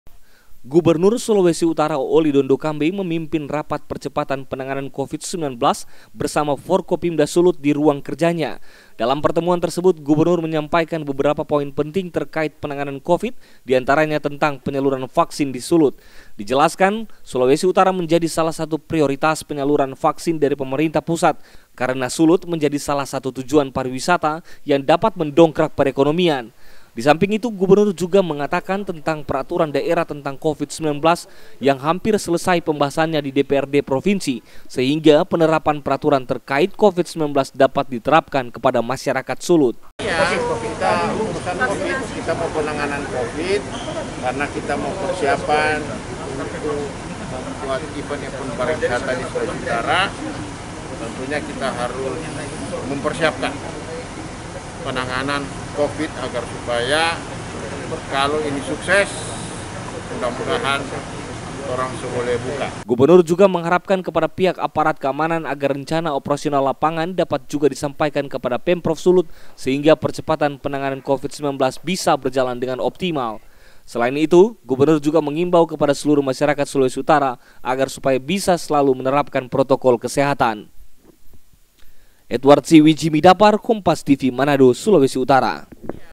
Gubernur & Forkopimda Sulut Gelar Rapat Percepatan Penanganan COVID-19
MANADO, KOMPAS.TV- Untuk lebih menekan angka penyebaran COVID-19, Gubernur Sulawesi Utara bersama Forum Kordinasi Pimpinan Daerah atau Forkopimda Sulawesi Utara menggelar rapat percepatan penanganan COVID-19 diruang rapat Gubernur Sulut.

Gubernur Sulawesi Utara Olly Dondokambey memimpin rapat percepatan penanganan COVID-19 bersama Forkopimda Sulut diruang kerjanya. Dalam pertemuan tersebut, Gubernur menyampaikan beberapa poin penting terkait penanganan COVID diantaranya tentang penyaluran vaksin di Sulut.

Dijelaskan juga bahwa, Sulawesi Utara menjadi salah satu prioritas penyaluran vaksin dari Pemerintah Pusat karena Sulut menjadi salah satu tujuan pariwisata yang dapat mendongkrak perekonomian.

Disamping itu, Gubernur juga mengatakan tentang peraturan daerah tentang COVID-19 yang hampir selesai pembahasannya di DPRD Provinsi, sehingga penerapan peraturan terkait COVID-19 dapat diterapkan kepada masyarakat Sulut.

Gubernur juga mengharapkan kepada pihak aparat keamanan agar rencana operasional lapangan dapat juga disampaikan kepada Pemprov Sulut sehingga percepatan penanganan COVID-19 bisa berjalan dengan optimal.

Selain itu, Gubernur juga menghimbau kepada seluruh masyarakat Sulawesi Utara agar supaya bisa selalu menerapkan protokol kesehatan, agar bisa bersama-sama memutus mata rantai penyebaran COVID-19 di Sulut.

Edward Siwi Jimmy Dapar Kompas tv Manado

Sulawesi Utara